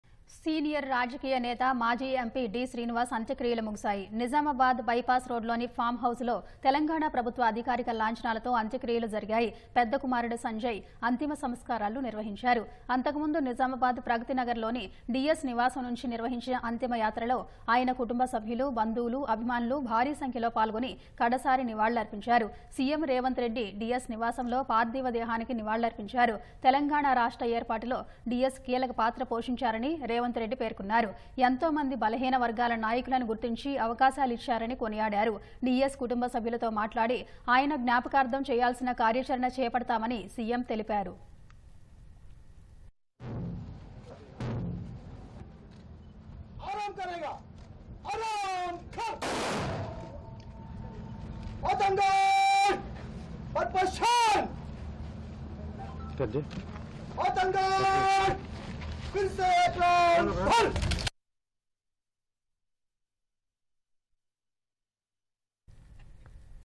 D. Srinivas Funerals Completed | Nizamabad Bypass Road | ముగిసిన డి.ఎస్ అంత్యక్రియలు
సీనియర్ రాజకీయ నేత, మాజీ ఎంపీ డి.శ్రీనివాస్ అంత్యక్రియలు ముగిశాయి. నిజామాబాద్ బైపాస్ రోడ్డులోని ఫామ్ హౌజ్ లో. తెలంగాణ ప్రభుత్వ అధికారిక లాంఛనాలతో అంత్యక్రియలు జరిగాయి. పెద్ద కుమారుడు సంజయ్ అంతిమ సంస్కారాలు నిర్వహించారు. అంతకుముందు. నిజామాబాద్ ప్రగతినగర్ లోని డీఎస్ నివాసం నుంచి నిర్వహించిన అంతిమయాత్రలో ఆయన కుటుంబసభ్యులు, బంధువులు, అభిమానులు భారీ సంఖ్యలో పాల్గొని. కడసారి నివాళులర్పించారు. సీఎం రేవంత్ రెడ్డి డీఎస్ నివాసంలో. పార్థివదేహానికి నివాళులర్పించారు. తెలంగాణ రాష్ట్ర ఏర్పాటులో డీఎస్ కీలకపాత్ర పోషించారని. రేవంత్ రెడ్డి పేర్కొన్నారు. ఎంతోమంది బలహీన వర్గాల నాయకులను గుర్తించి. అవకాశాలిచ్చారని కొనియాడారు. DS కుటుంబ సభ్యులతో మాట్లాడి. ఆయన జ్ఞాపకార్థం చేయాల్సిన కార్యాచరణ చేపడతామని సీఎం తెలిపారు.